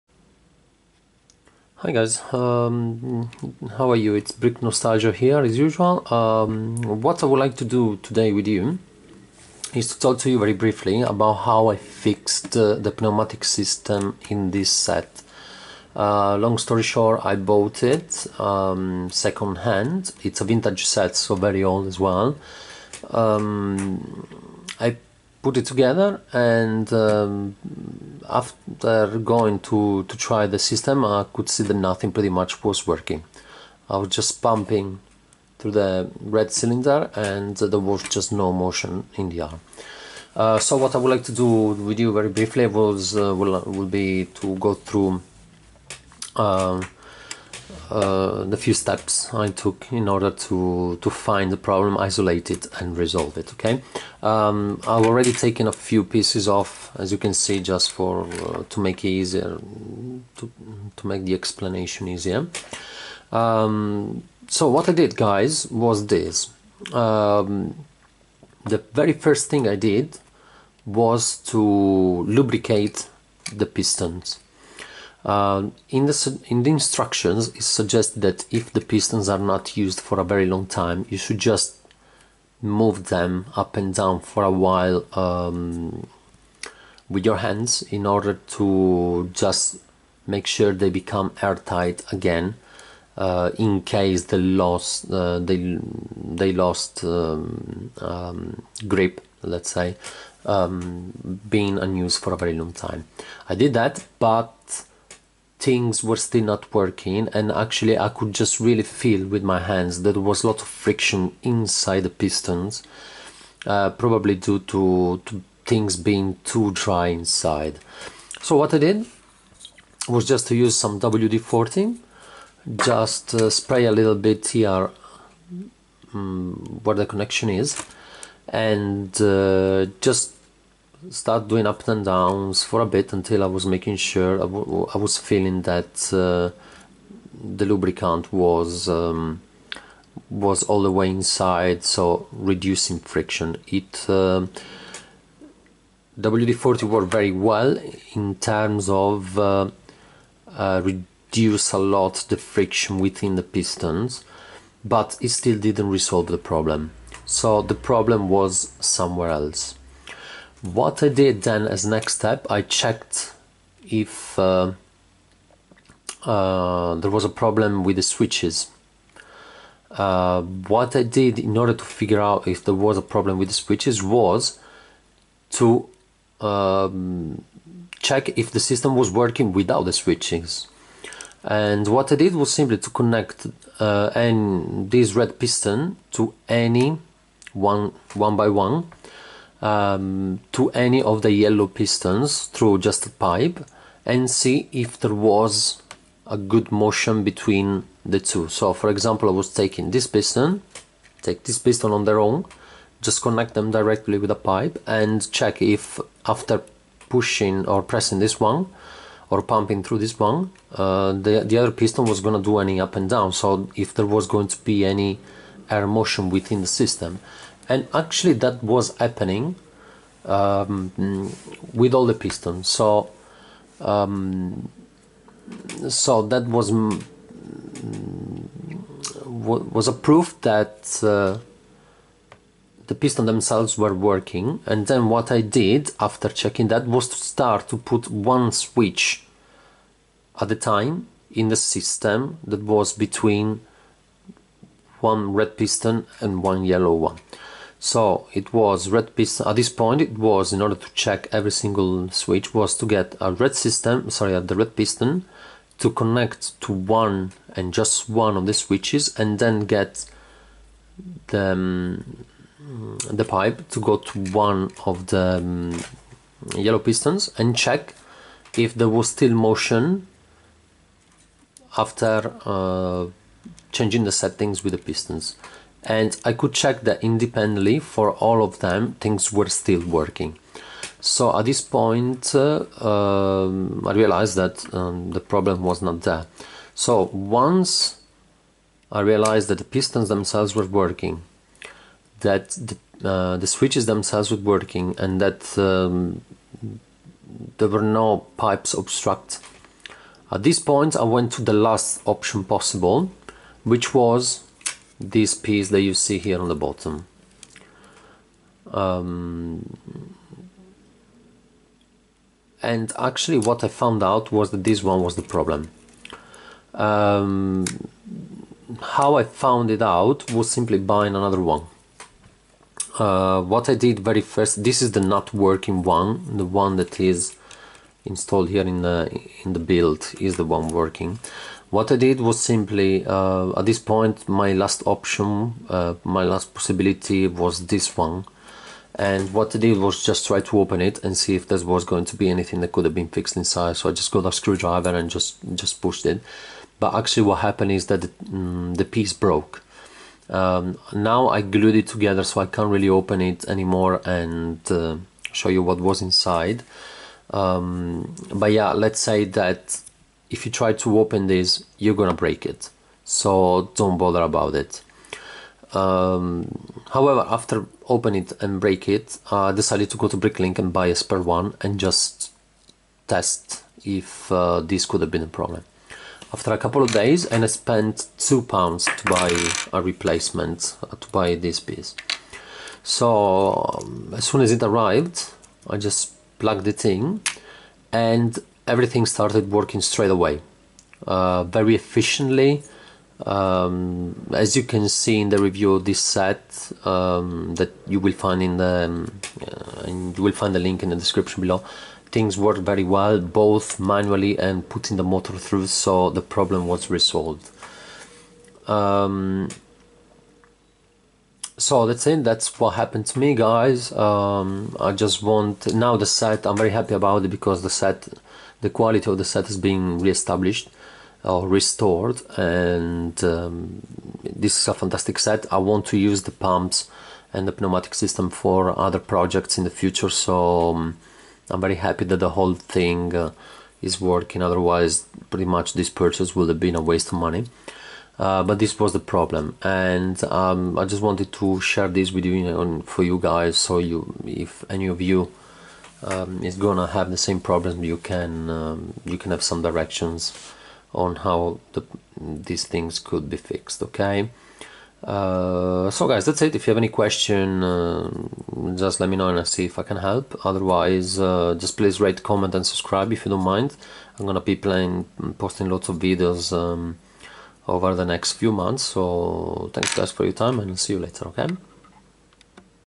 How To Fix The Pneumatic System Of A Lego Technic Set
Hey guys,

in this video I'm going through the steps that allowed me to fix my vintage 8851 technic set...I hope it helps!